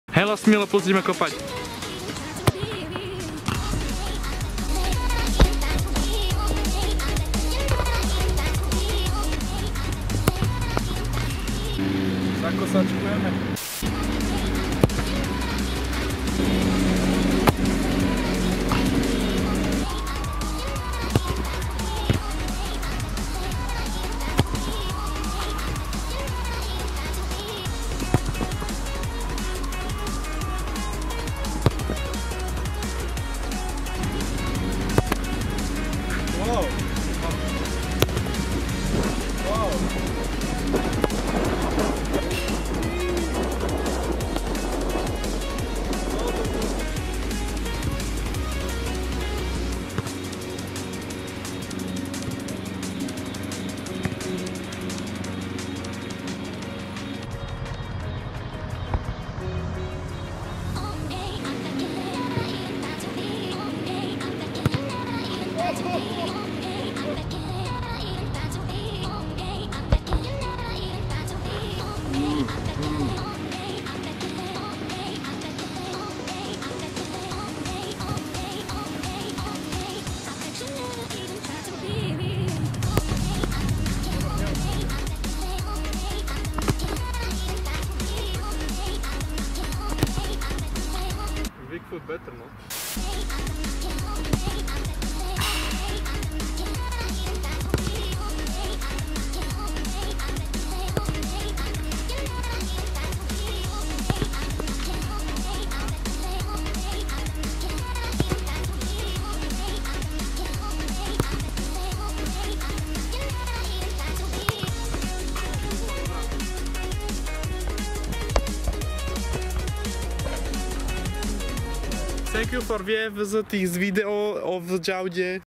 Epické Strieľanie na Bránu!
Dnes to bude zostrih z krátkeho kopkanya na ihrisku (づ｡◕‿‿◕｡)づ. Vo videu uvidíte rôzne spôsoby strelby na bránu. Aké by ste chceli také podobné krátke video?